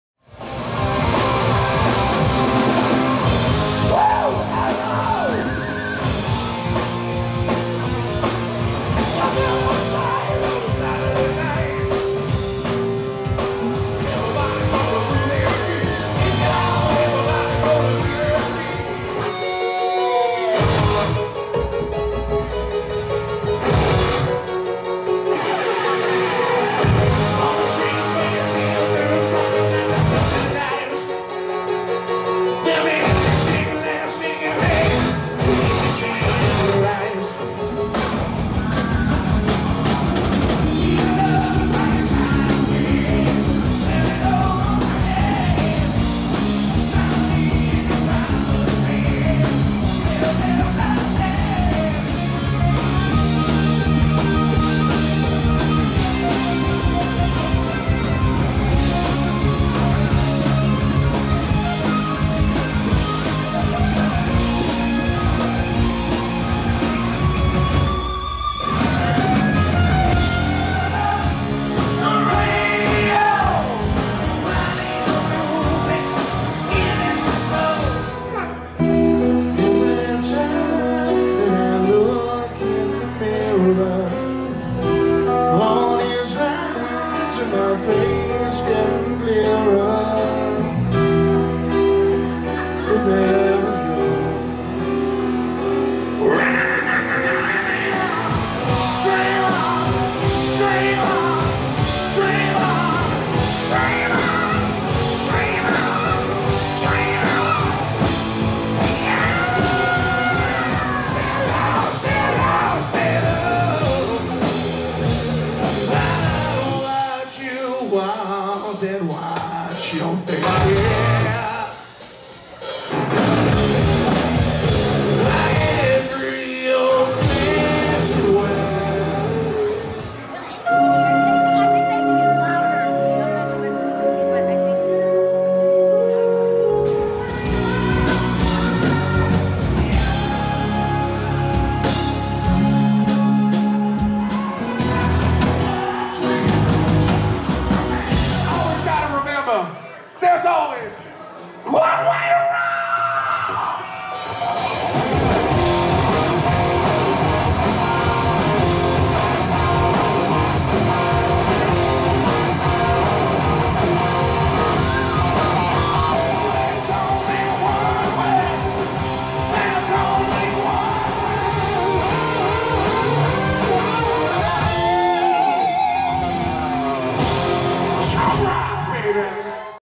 Hard Vinyl Returns to Tailgaters 1-31-20
Hard Vinyl returns to Tailgater on 1-13-20 a Chicago classic rock cover band featuring John Yeager on Lead Vocals, Ric Cwiak on lead guitar, Tom Shimko on rhythm guitar, Mike Carbonara on keyboards, Brian Madzurak on drums and Rob Graf on Bass,